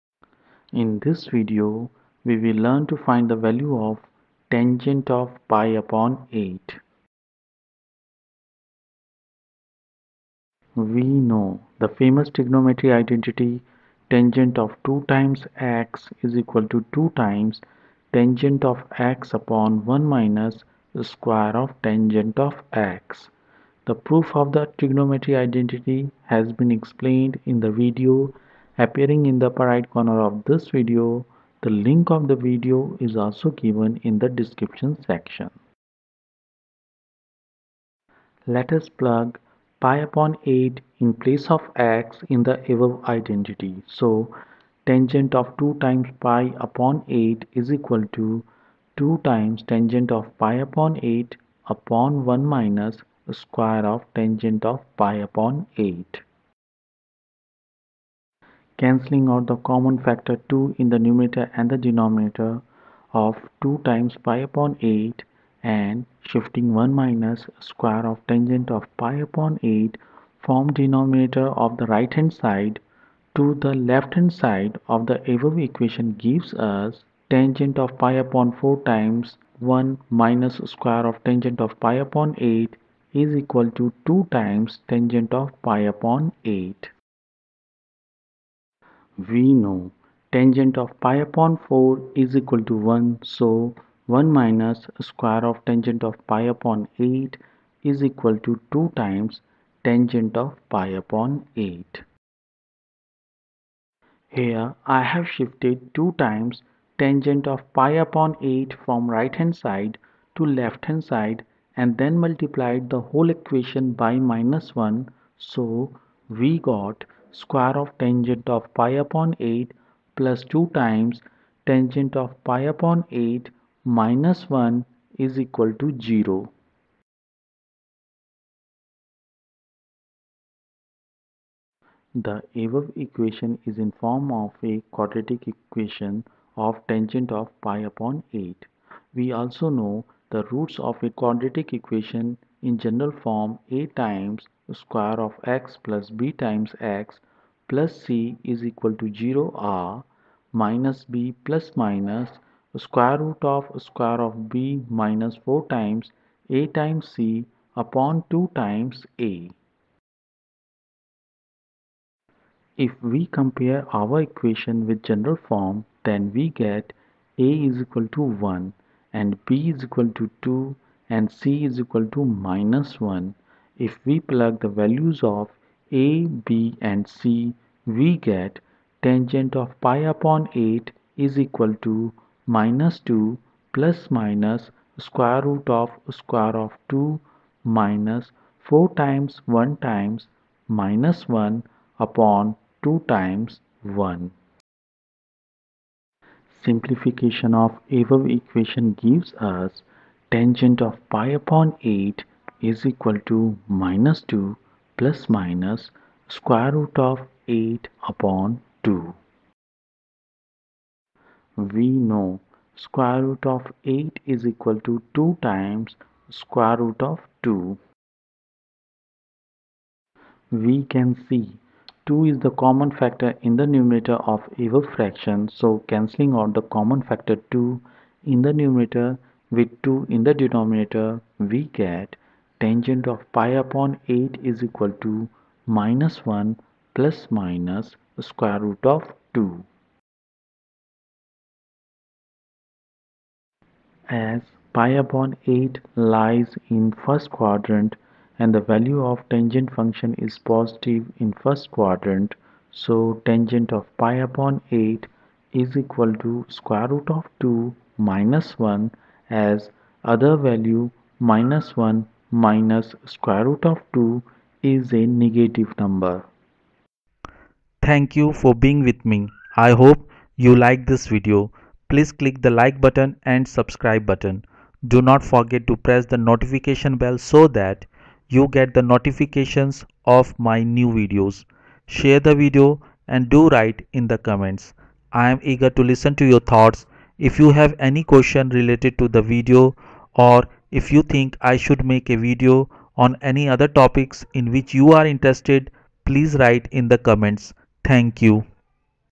tan(pi/8)
In this video, we will learn to find the value of tan(pi/8).

The link of the video explaining the proof of the identity tan 2x = (2 tan x)/(1 – tan^2 x) has been given below:

Other topics of the video:
Value of tan(pi/8)
Find the exact value of tan(pi/8)
What is the exact value of tan(pi/8)?